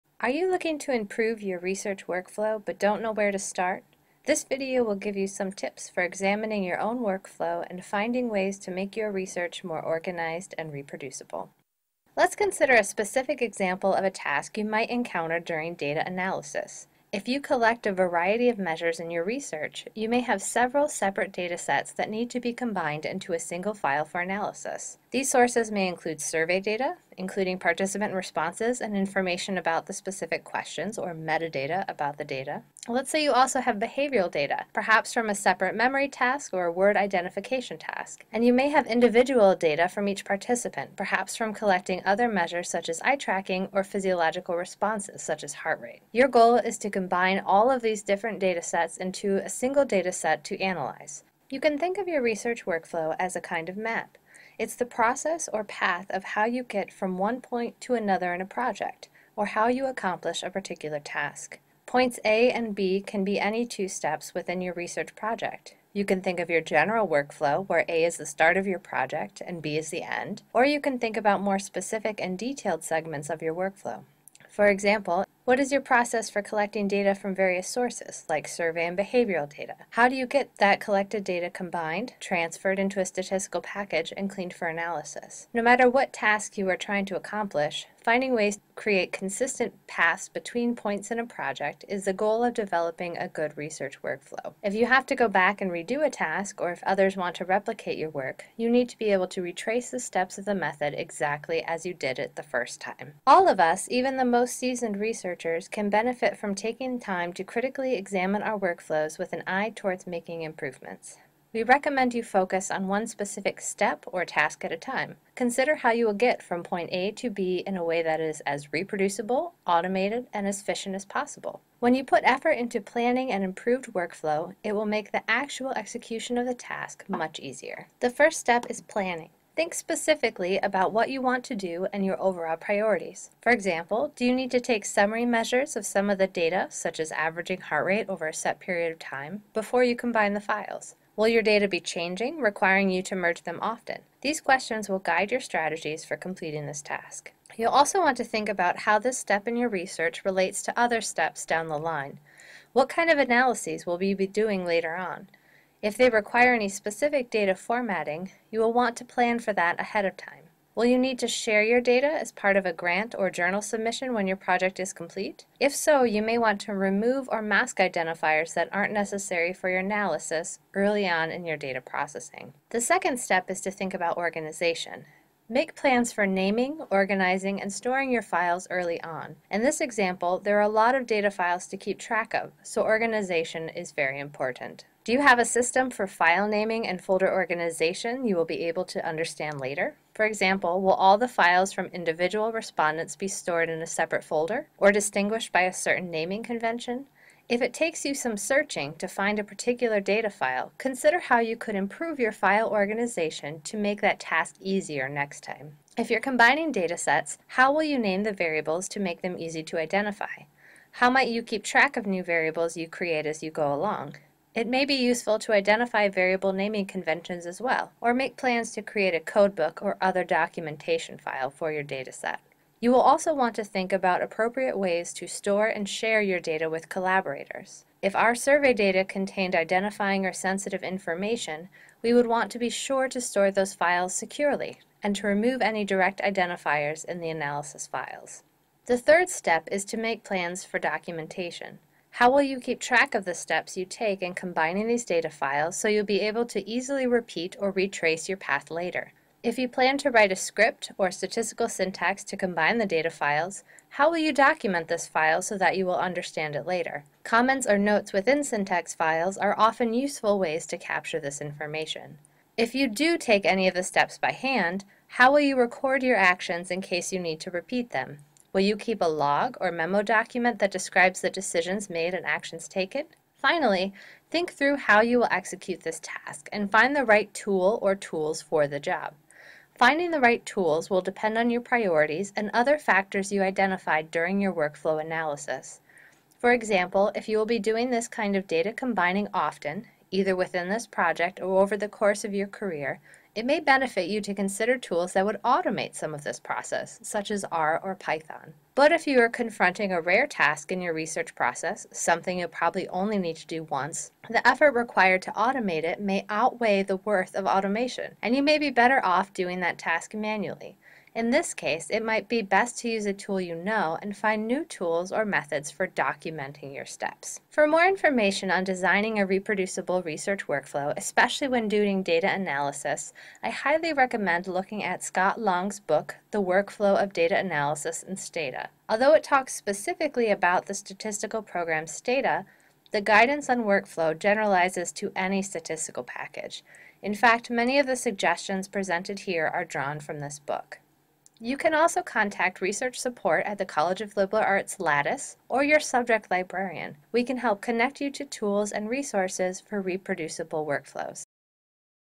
How To Perform A Workflow Analysis
Are you looking to improve your research workflow, but don’t know where to start? This video will give you some tips for examining your own workflow and finding ways to make your research more organized and reproducible.